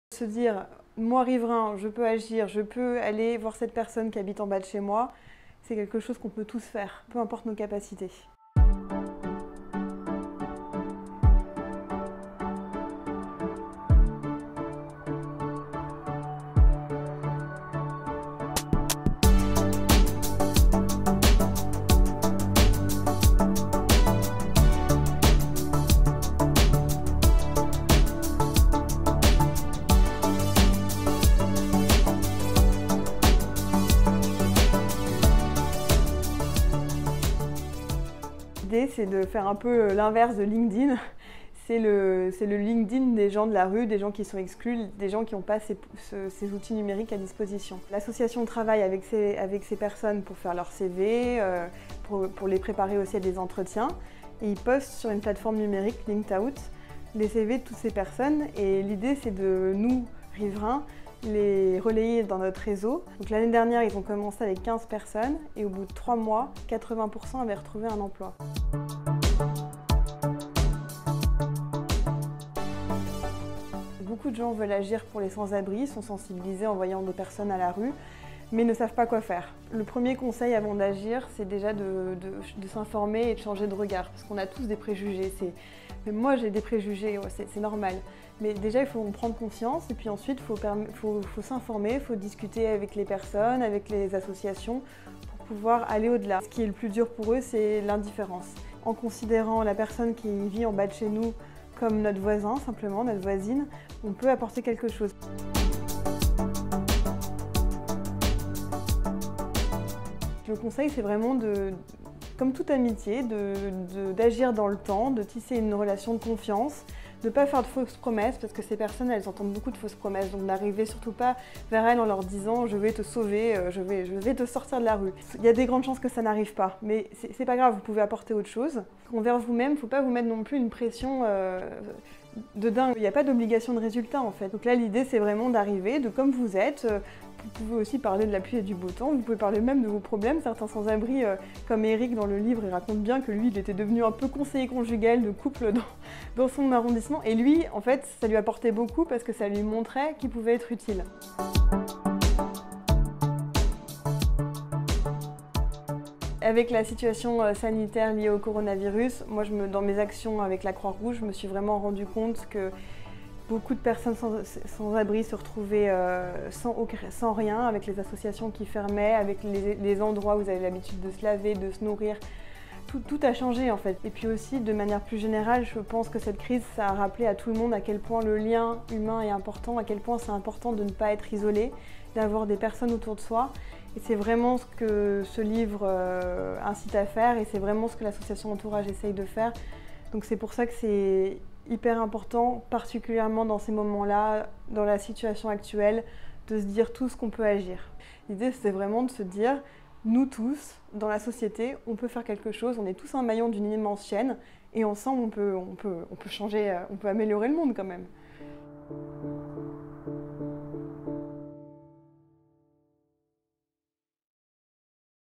Retisser du lien entre personnes sans-abri et riverains
Dans son ouvrage "Avec et sans-abri, le guide pour tisser du lien", co-édité par l'association Entourage, la journaliste Lauriane Clément livre ses conseils pour oser s'adresser aux personnes sans-abri, en les considérant simplement comme un voisin ou une voisine du quartier.

Retrouvez plus d'infos sur WeDemain.fr